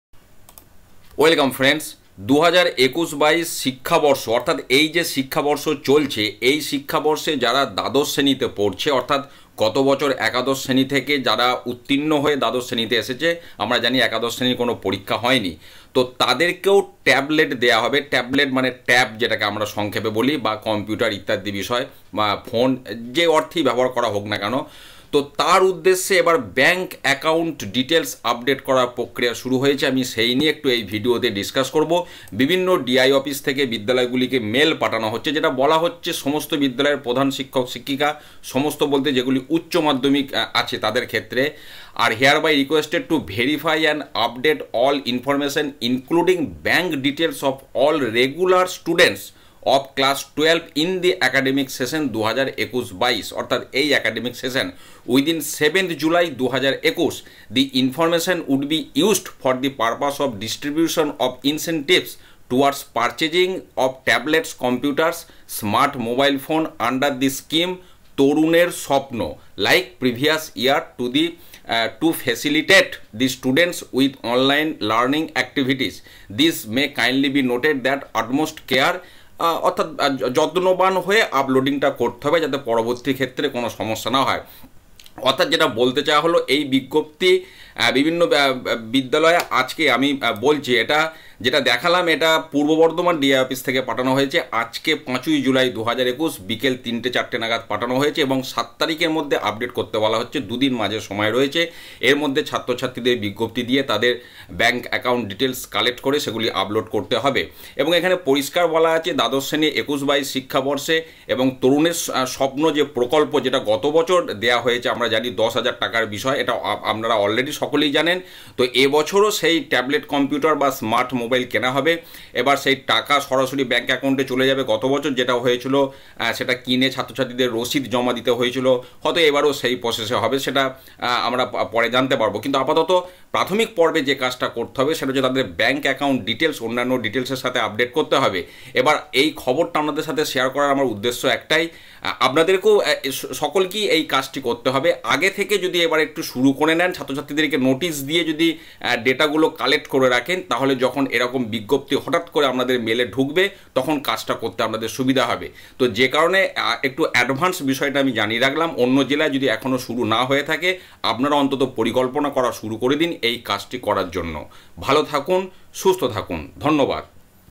Class XII, 2021-2022 Session সকলকে TAB দেওয়া হবে, বিজ্ঞপ্তি প্রকাশিত হল
Incentive TAB for all Class-XII students in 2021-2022,
Date : 05/07/2021